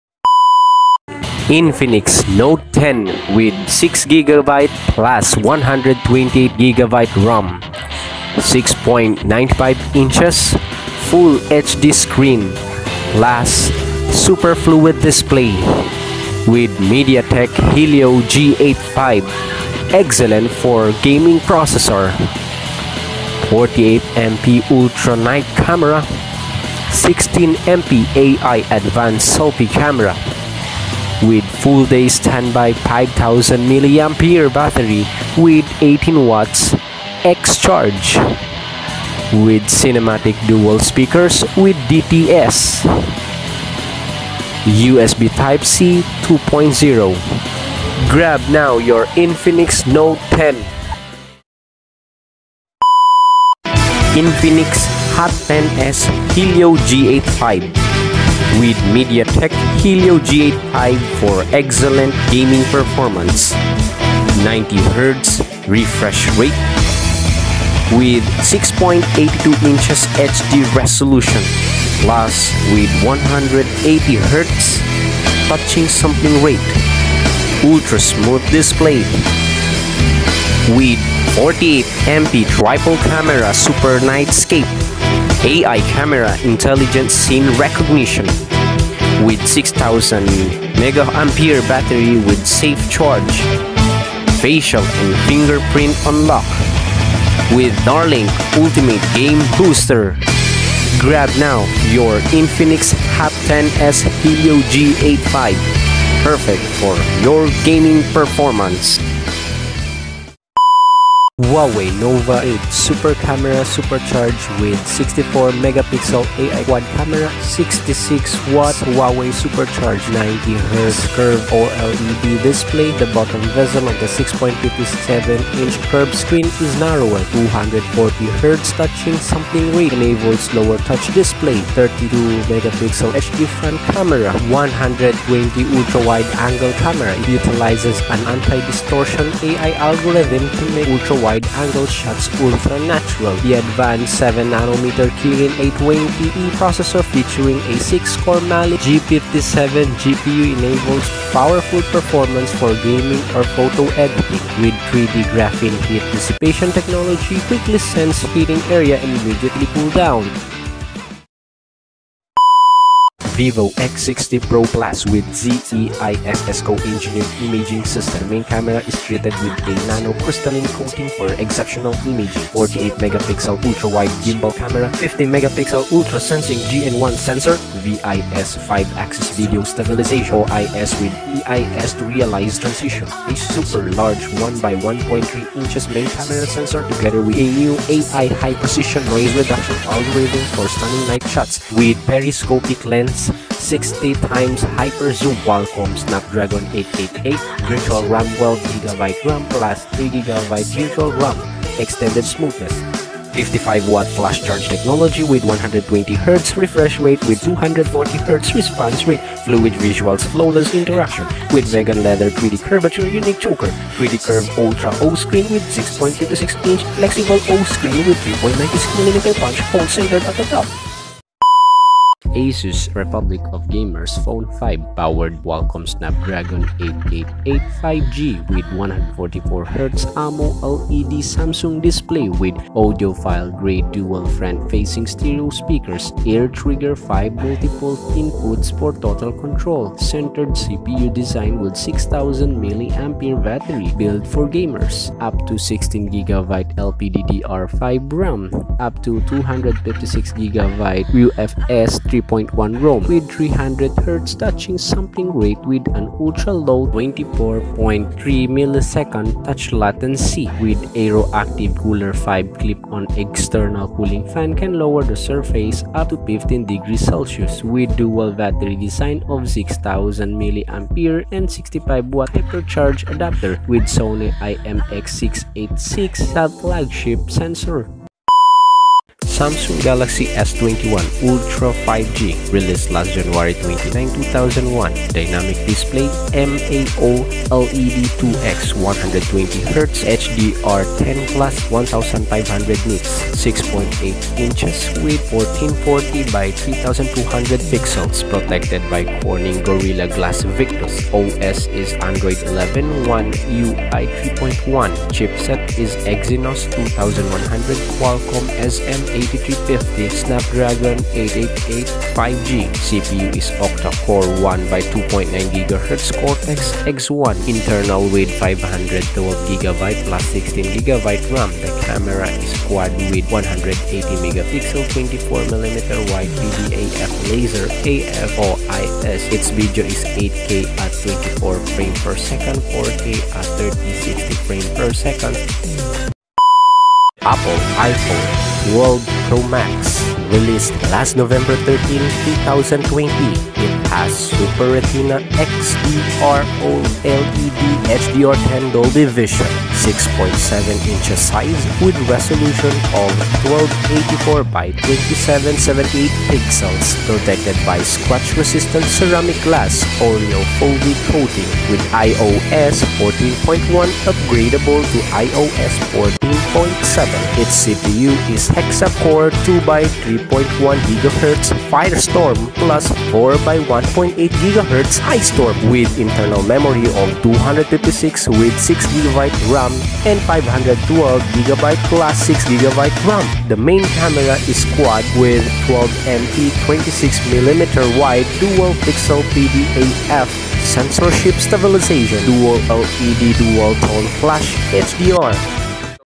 Top Phones That You Should Have This 2021: What Do These Phone Have in Gaming and Photography?
These Phones are Monster in Gaming, High MP and Long Lasting Batteries!!

6.7" size, with resolution of 1284 x 2778 pixels
protected by scratch-resistant ceramic glass, oleophobic coating
with iOS 14.1 upgradable to iOS 14.7
CPU is Hea core 2x 3.1 GHz Firestorm + 4 x 1.8 GHZ Icestrom
Internal memory is 512 GB+ 6GB RAM
Main Camera is Quad 12 MP 23 mm SL 3D depth and Biometric sensor
4k @ 24/30/60fps 1080p @30/60/120fps gyro EIS.

6.8-inch Edge QHD+ Dynamic AMOLED 2X
Infinity-O Display (3200x1440), 515ppi, HDR10+ certified Adaptive 120Hz refresh rate (10~120Hz)
Eye Comfort Shield
Android 11
5nm Processor
2.9GHz (Maximum Clock Speed) + 2.8GHz +2.2GHz
12GB/16GG RAM, 256GB/512GB ROM
Rear Camera:12MP Dual Pixel AF (Ultra Wide), 108 MP Phase Detection AF(Wide, 10MP Dual Pixel AF (Tele 1), 10MP Dual Pixel AF (Tele 2)
Front Camera : 40MP Phase Detection AF
Ultrasonic Fingerprint sensor, Accelerometer, Barometer, Gyro sensor, Geomagnetic sensor, Hall sensor, Proximity sensor, Ambient Light Sensor
5,000 mAH Battery
WiFi + LTE + 5G
Bluetooth v5.0
Water and Dust Resistance
Face Recognition, Ultrasonic Fingerprint Sensor

6.95" FHD screem plus super fluid display
Mediateck Helio G85 Excellent Gaming Processor
48MP Ultra Night Camera
16 MP AI Advanced Selfie Camera
Full day Standby 5000 mAh battery with 18 W X-charge
cinematic Dual speakers with DTS.

ZEISS co-engineered Imaing System. The Main Camera is treated with a Nanocrystalline coating for exceptional imaging
48 MP Ultra wide Gimbal Camera
50 MP Ultra sensing Gn1 Sensor
VIS 5-axis video stabilizatio
OIS with EIS to realize transition
1 X 1.3" main camera sensor with new AI high precision noise reduction algorithm.
With periscopic lens/60x Hyper Zoom.
Qualcomm, Snapdragon 888
Virtual RAM 12 Gb + 3GB Extended Smoothness
55 W flash charge technology
120 Hz Refresh Rate with 240 Hz repsonse rate fluid visuals, flawless interactions
With Vegan leather, 3D curvature uniquechoker
3D curved Ultra O screen with 6.56" flexible O screen with 3.96 mm punch hole centered at the top.

powered by Qualcomm Snapdragon 888 5G with 144Hz AMOLED Samsung Display
Audiophile-Grade Dual Front facing stereo-speakers
Air trigger 5 Multiple inputs for total control centered CPU design with 6000 mAh baterry
Up to 25% faster Cpu performance , Upto 35 % faster GPU performance, Upto 16GB LPDDR 5 RAM
Up to 256 256 GB UFS 3.1 ROM
with 300Hz touching sampling rate with an ultra low 24.3ms touch latency
with Aero active cooler 5 clip-on external cooling fan canlower the surface temperature near the CPU by up to a stunning 15 degrees Celsius.
with DUal battery design of 6000mAh battery and 65 W Hyoer charge Adapter.
With SONY IMX 686 flagshiip sensor
industry leading 64 MP wide angle camera wth pro video mode.

Huawei Nova 8 2021
Super Camera Super Charge
64MP AI Quad Camera and 66W Huawei super charge
90 HZ curved OLED Display
6.57" curved screen
240 Hz touching sampling rate
32MP HD Front Camera
120 ultra wide angle camera
Anti-distortion AI Algorithm to make ultra wide angle shots ultra natural
Advanced 7nm Kirin 820E processor featuring 6-core Mali-G57 GPU enables powerful performance for gaming or photo editing.
With 3D Graphene heat dissipation technology, quickly sense heating area and immediately cool downs.